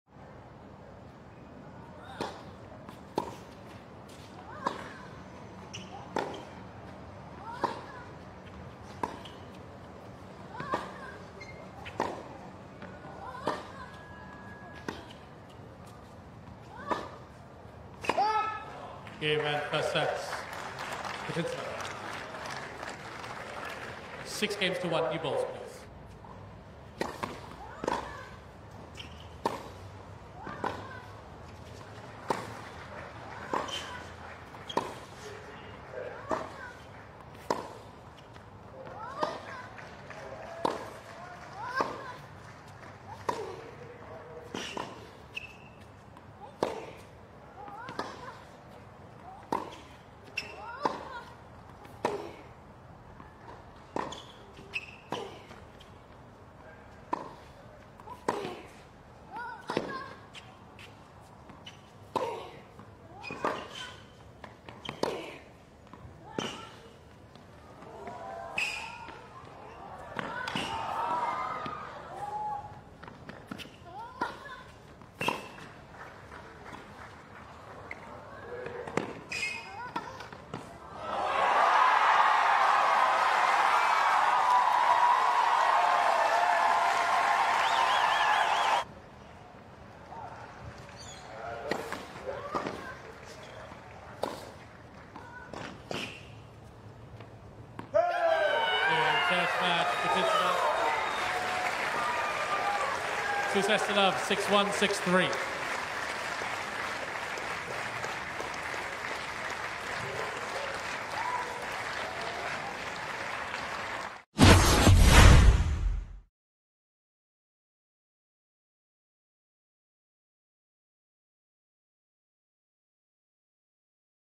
Yulia Putintseva vs. Su-Wei Hsieh - Match Highlights (1R) | Australian Open 2020
Watch the Match Highlights from Yulia Putintseva vs. Su-Wei Hsieh, 01/21/2020